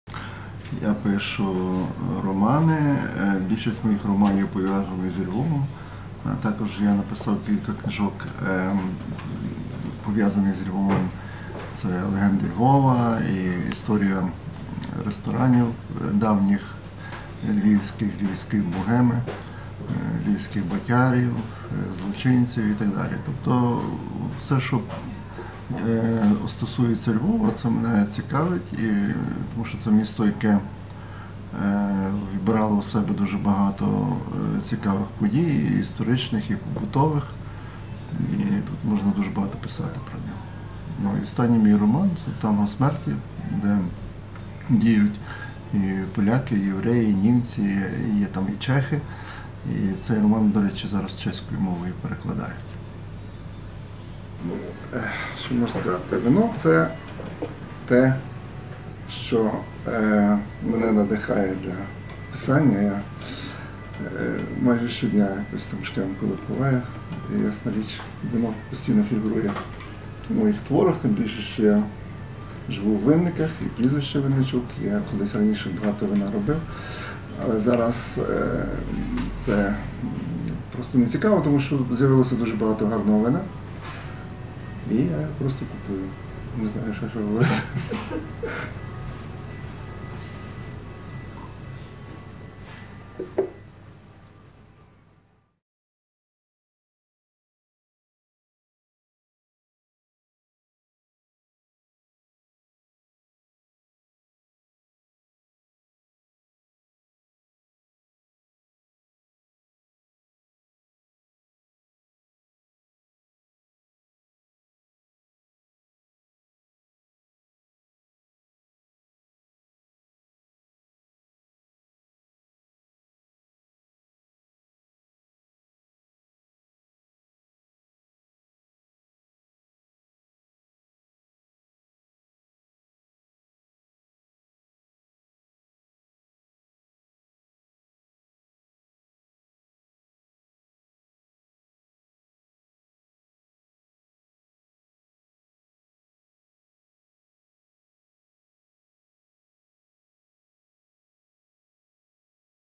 Jurij Vynnyčuk / Юрій Винничук — MAČ, mimořádná edice: Ukrajina
Narodil se roku 1952 ve Stanislavově, pozdějším Ivano-Frankivsku. Prozaik (erotický román Jarní hry v podzimních sadech, 2005; román Hrušky v těstě, 2010; česky vyšla antologie jeho surrealisticky absurdních povídek Chachacha, 2009 a také kniha lvovských pověstí Příběhy z Haliče, 2002), novinář, redaktor, překladatel (mimo jiné přeložil některá díla Bohumila Hrabala) a básník. Byl také režisérem lvovského estrádního divadla. Proslul jako autor literárních mystifikací, mimo jiné ukrajinského překladu smyšleného irského eposu. Ve svém posledním románu Tango smrti (2012), opírajícím se o legendu z janivského koncentračního tábora, spojuje vyprávění o Haliči v meziválečné a válečné době s naší současností. Básník Andrij Ljubka prohlásil, že „od okamžiku, kdy bylo napsáno Tango smrti, máme ukrajinského Umberta Eca, v němž je navíc trochu Borgese“.

Realizováno s finanční podporou Statutárního města Brna v rámci projektu Měsíc autorského čtení 2022, mimořádná edice: Ukrajina.